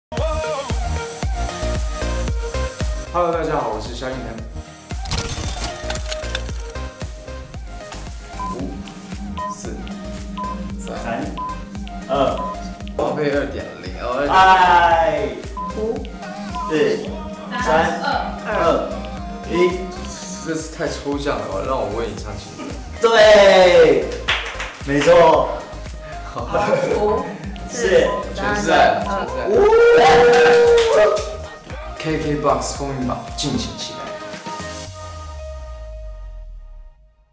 【限時五秒超樂想像】蕭敬騰挑戰看畫猜歌！反應居然是...... - 第 14 屆 KKBOX 風雲榜
你對 蕭敬騰 的歌熟嗎？考驗是不是鐵粉的時候到啦！
覺得第二首有點難但小Ｋ還是秒答了😉

再看一次
分享